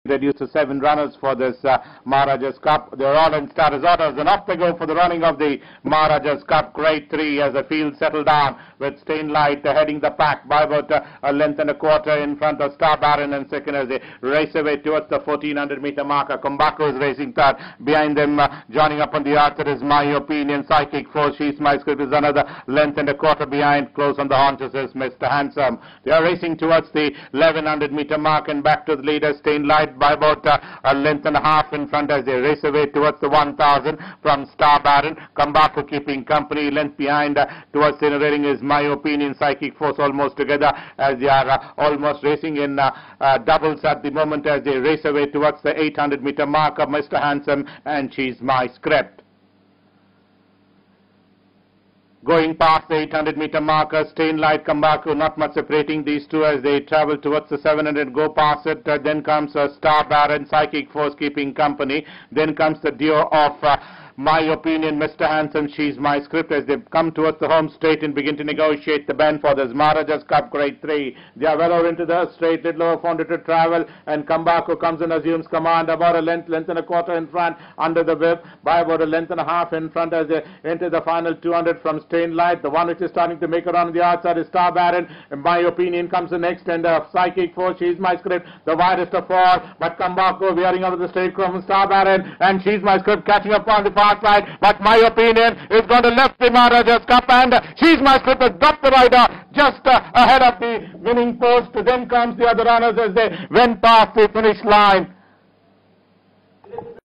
The Maharaja's Cup (Gr.3) WINNER: KAMBAKU
Leading trainer Arjun Manglorkar understandably was down after Thailand performed below par in the 1000 Guineas on Thursday. The wheel of fortune swung in his favour the next day, lesser fancied Kambaku emerging the new Maharaja, winning the prestigious Maharaja's Cup after the Chennai  star My Opinion  was disqualified  after winning the event. My Opinion from trainer B Suresh's yard  shifted out and  and interfered with the progress of Shesmyscript who was making a run on the outside and as result it appeared that the Irfan ward Shesmyscript clipped the heels of the winner and Trevor Patel astride Shesmyscript was unseated. The stewards decided to disqualify My Opinion and since Shesmyscript dropped her jockey before the winning post, Kambaku who had finished second was declared the winner to the delight of Arjun while the Chennai camp was not pleased pointing out that it was Shesmyscript, who had clipped the winner. But the fact that My Opinion shifted out at the all-important end resulting in a fall went against Imran and My Opinion. In racing it is not your opinion or my opinion which counts and only the opinion of stewards holds good  and Kambaku  was  declared the new Maharaja. Psychic Force was placed second while Star Baron, the stablemate of My Opinion took the third spot.